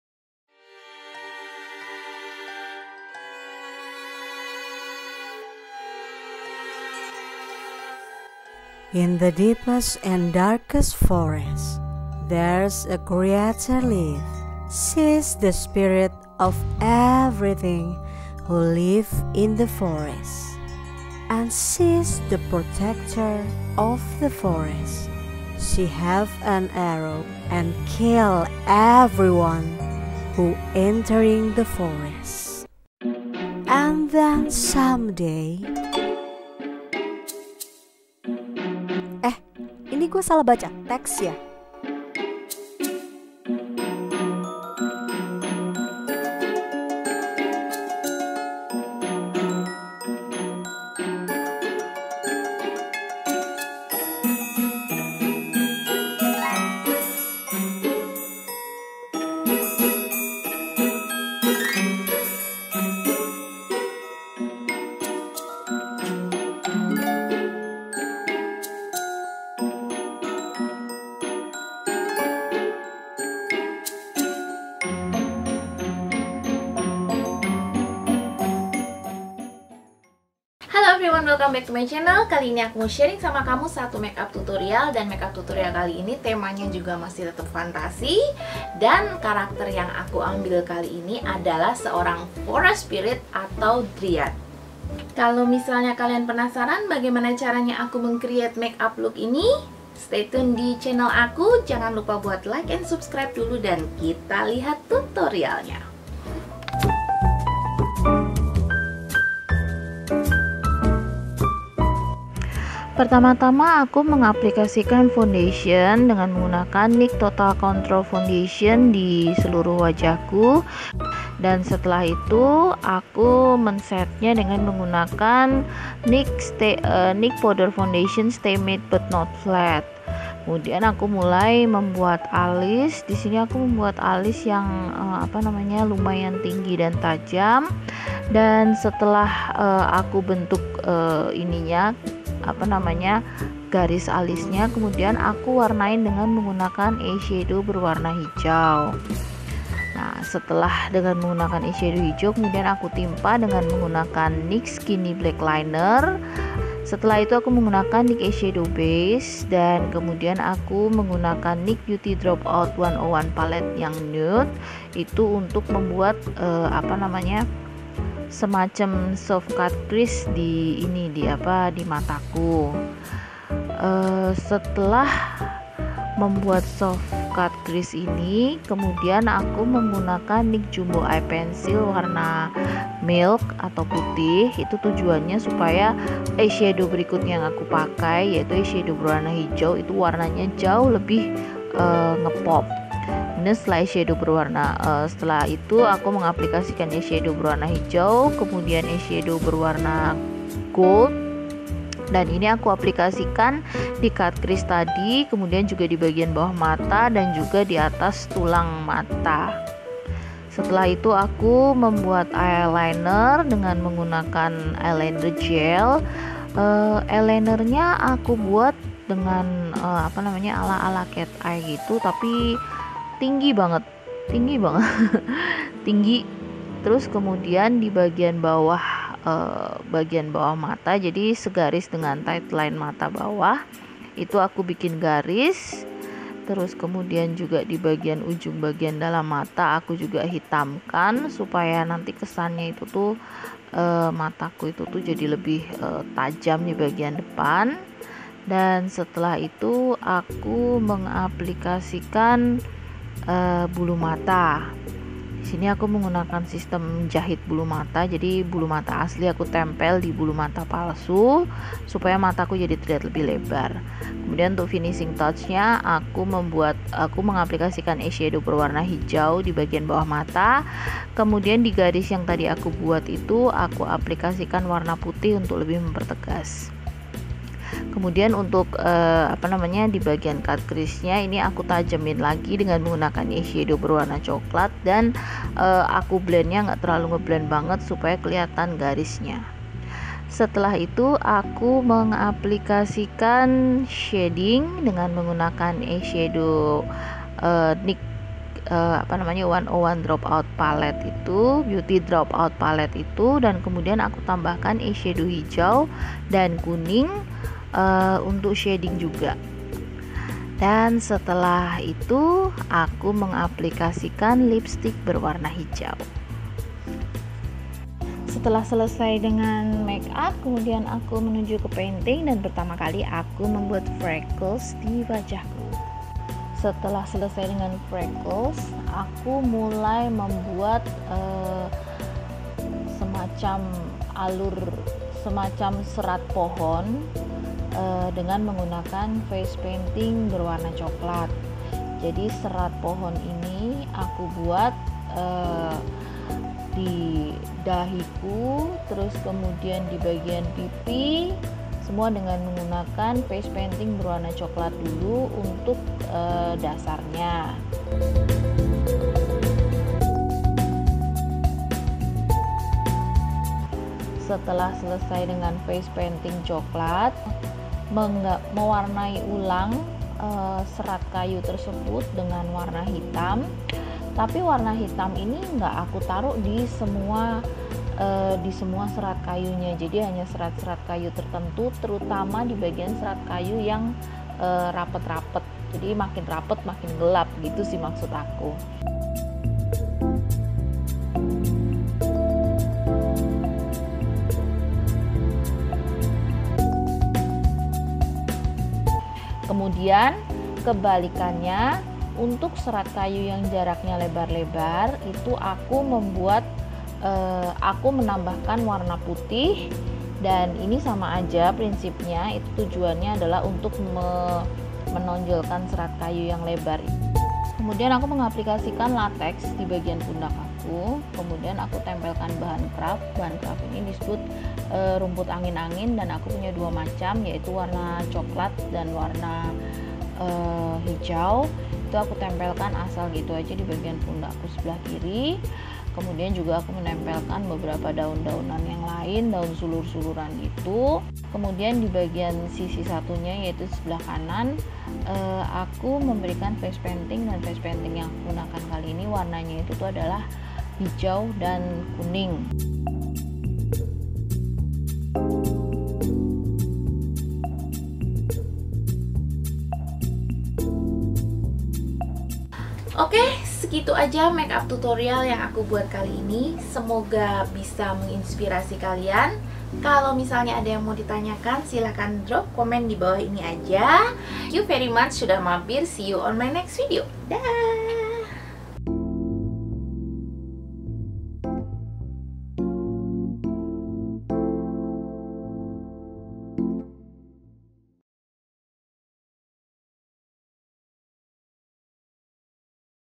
Poppiri The Forest Spirit || NYX Face Award Indonesia 2017 Entry
Baca Informasi disini :

Video ini merupakan entry kedua aku untuk NYX Face Award 2017 Indonesia yang diadakan oleh NYX Cosmetics Indonesia. Karakter dalam video ini, merupakan remake dari Karakter yang pernah aku buat sebelumnya di Desember 2017. Semoga Menginspirasi.